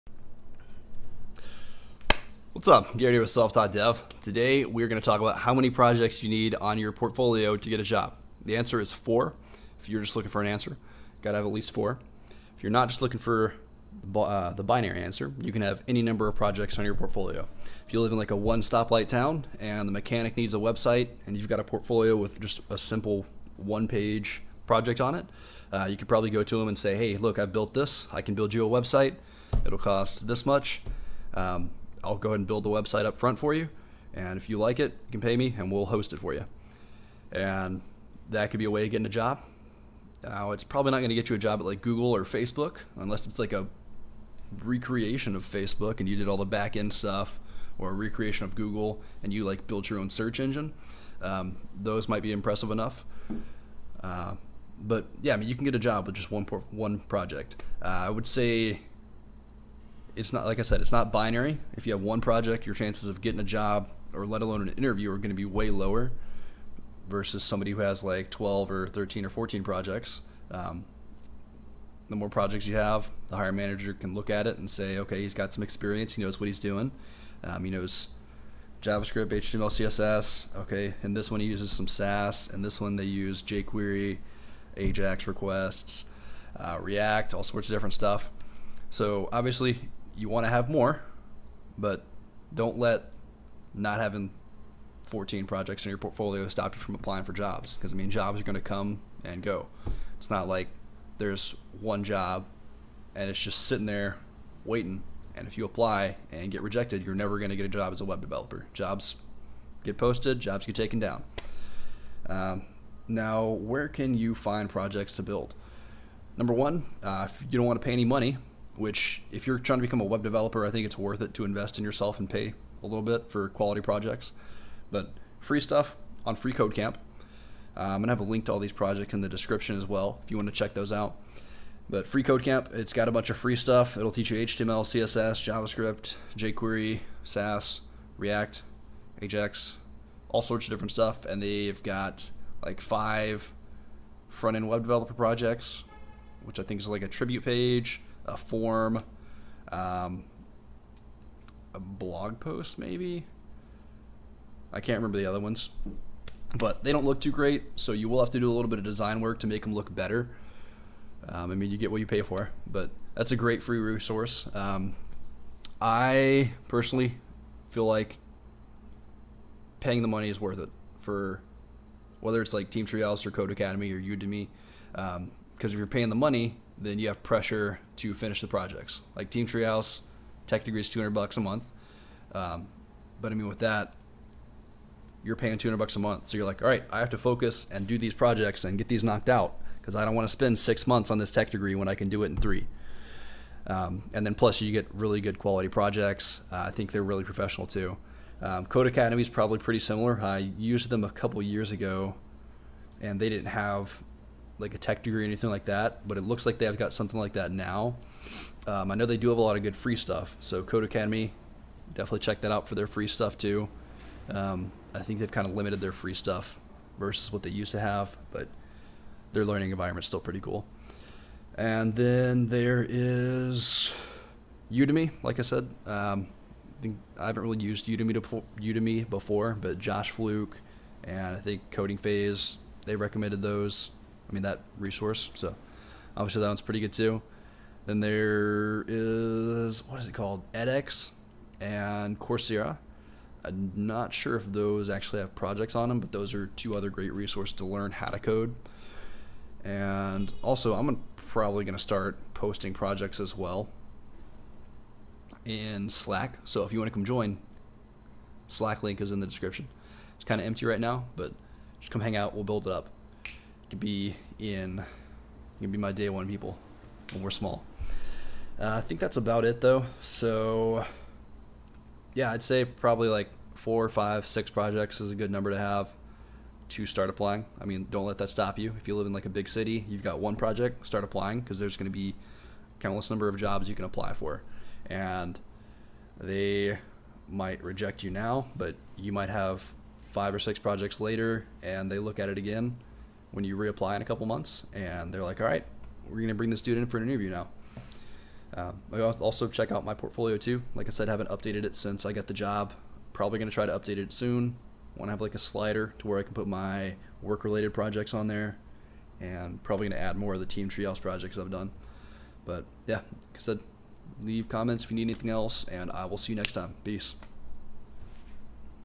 How Many Projects Do You Need On Your Portfolio?
How many projects do you need on your portfolio to start applying for web developer jobs?